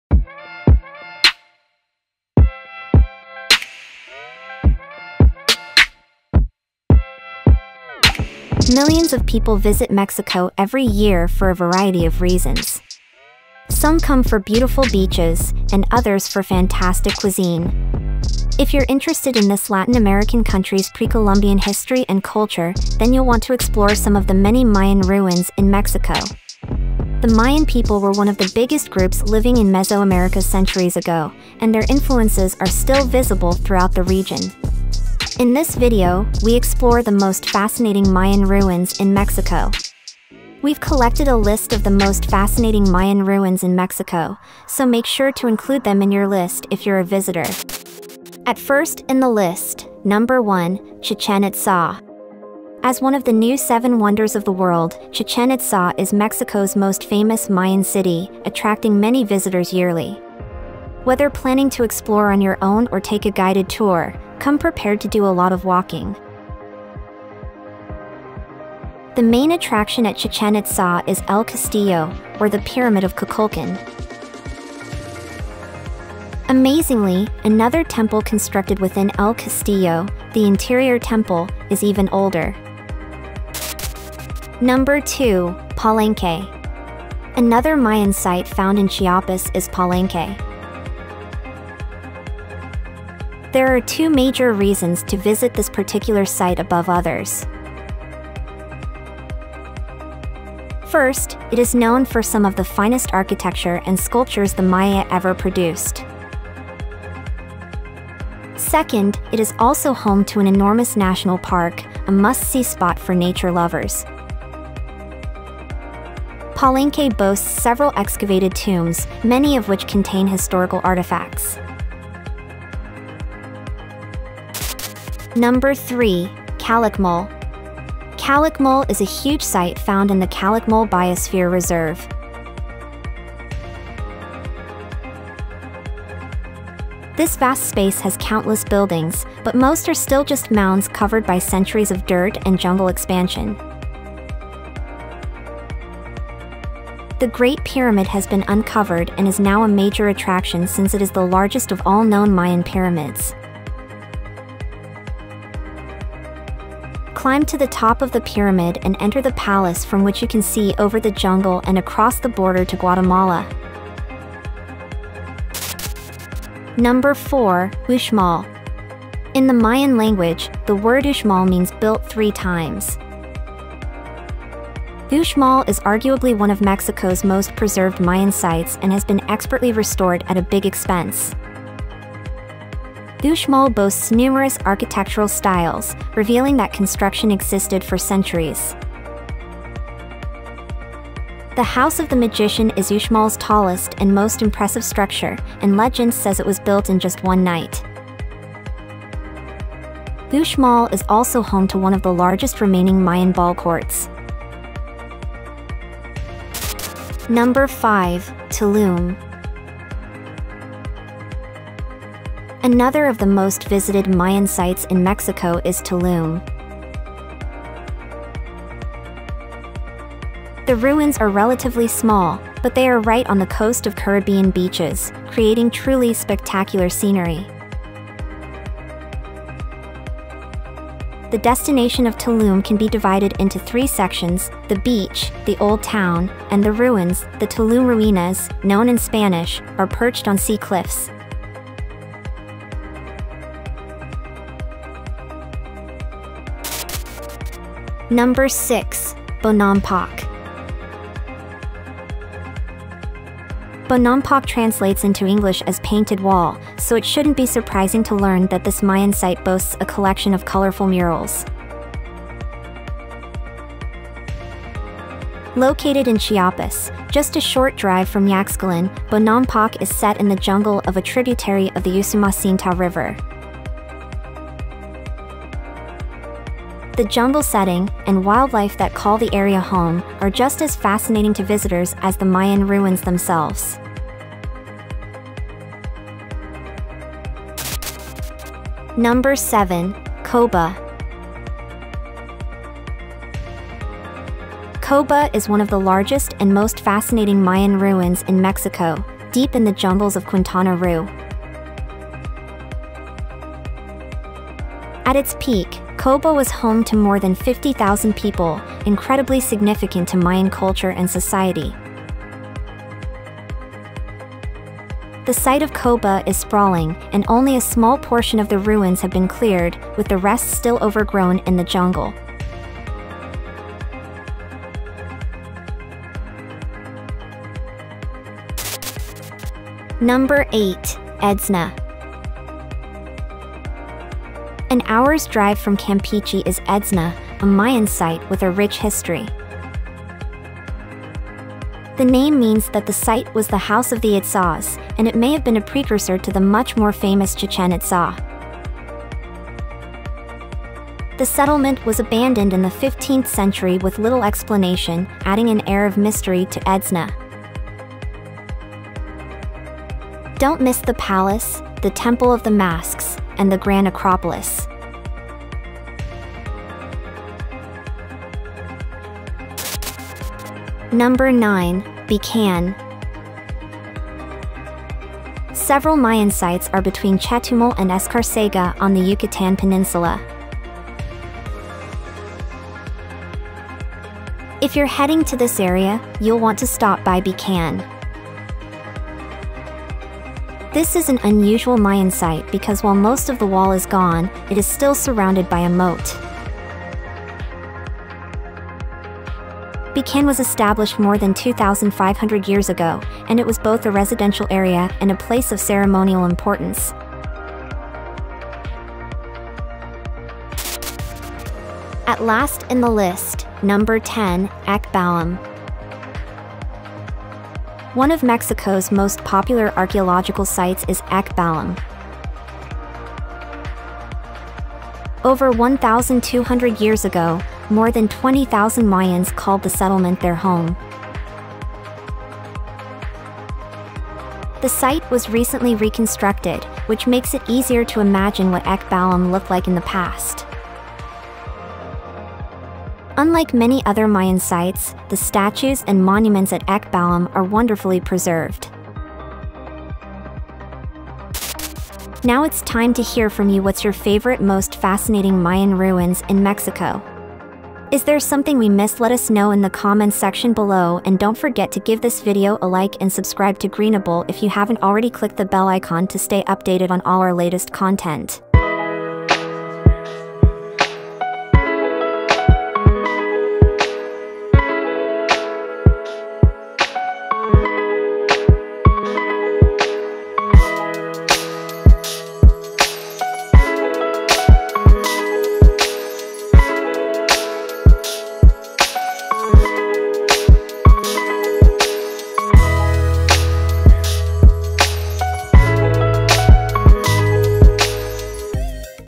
Top 10 Most Fascinating Mayan Ruins in Mexico - Travel Video 2023
Welcome to our amazing journey through the most captivating Mayan ruins in Mexico! From the mystical ruins of Chichen Itza to the breathtaking beauty of Tulum, we unravel the secrets and legacies of these ancient Mesoamerican civilizations. Prepare to be awestruck as we explore the architectural marvels, vibrant histories, and intriguing mysteries surrounding these sacred sites. Immerse yourself in the enchanting world of the Mayans, marvel at their astronomical knowledge, and witness the remnants of their advanced civilization. Join us on this unforgettable adventure through the fascinating Mayan ruins that showcase the rich cultural heritage of Mexico.

Discover the importance of these ruins within the Mayan civilization, learning about their religious practices, social structures, and advanced engineering skills demonstrated through majestic pyramids, temples, and stone carvings. Unravel the mysteries behind the astronomical alignment of these ruins with celestial bodies and the significance these celestial connections held for the Mayans. Through stunning visuals and insightful commentary, we bring to life the splendor and significance of sites like Palenque, Uxmal, Tikal, and more.

Chapters:

00:00 Introduction
00:47 Chichen Itza
01:23 Palenque
02:04 Calakmul
02:45 Uxmal
03:26 Tulum
04:07 Bonampak
04:48 Coba
05:29 Edzna
06:10 Becan
06:51 Ek Balam